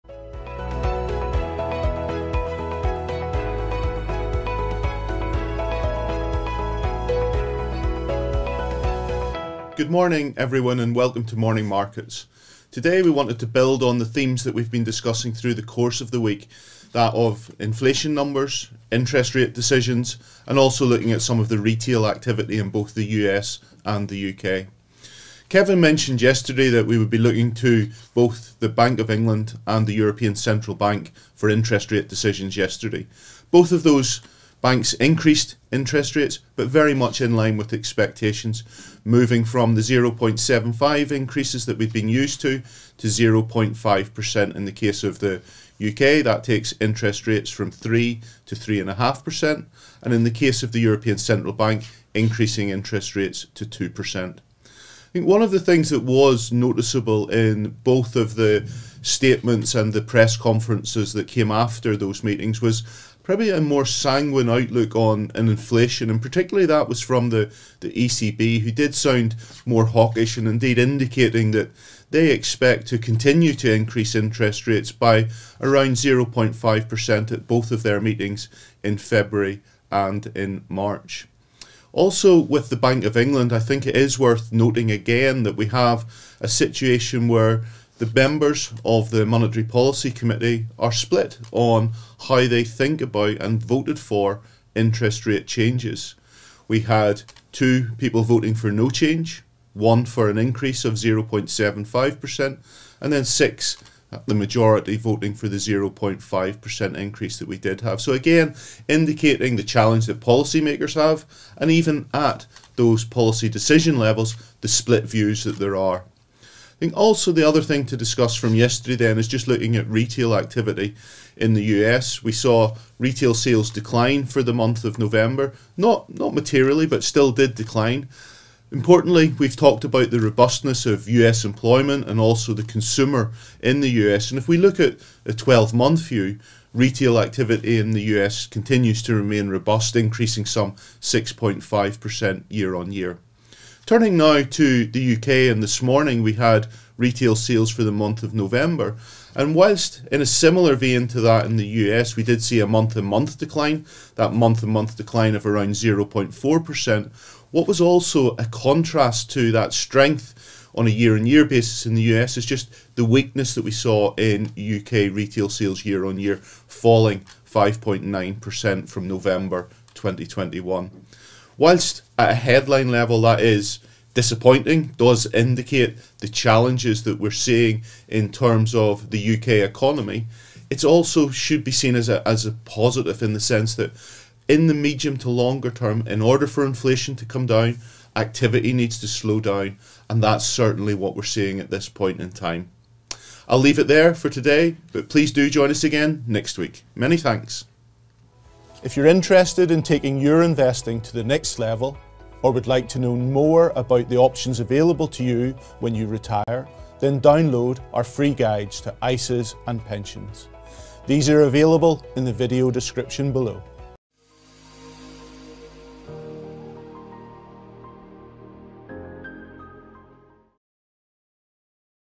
Interest rates increase | Morning Markets
On today's Morning Markets, Jeff Casson, Chief Investment Officer, reports on the increase in interest rates and what this means for markets and investors.

If you would like to download our guides to ISAs and Pensions, please visit the following